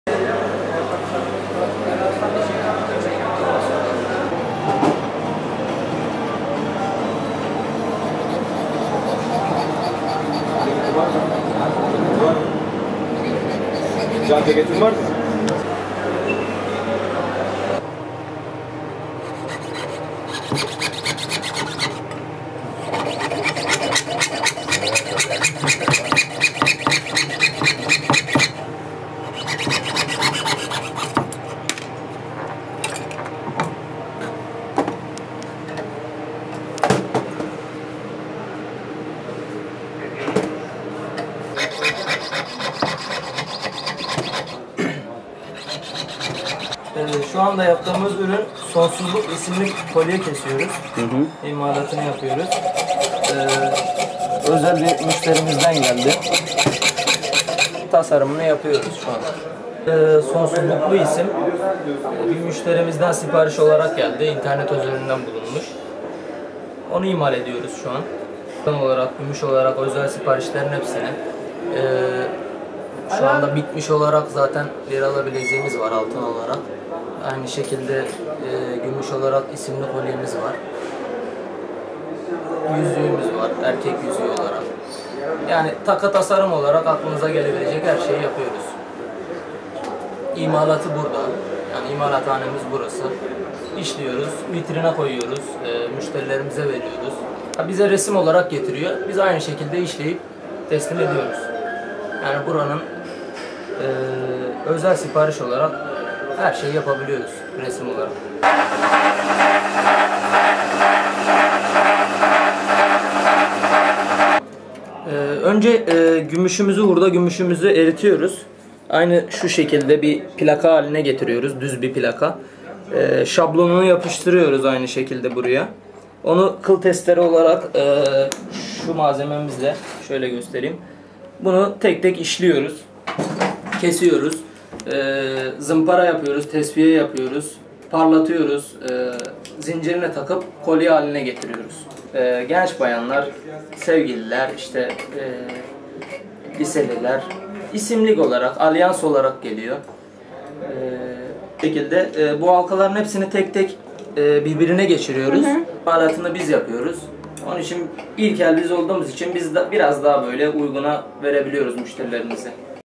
Altın ve Gümüş Tasarımında Ünü İskenderun’u Aştı!
Hatay’ın İskenderun İlçesinde çırak olarak başladığı Kuyumculuk mesleğinde kendini geliştirerek atölye açan Yusufcan Karacan yaptığı altın ve gümüş takı tasarımlarında ünü kent dışına taştı.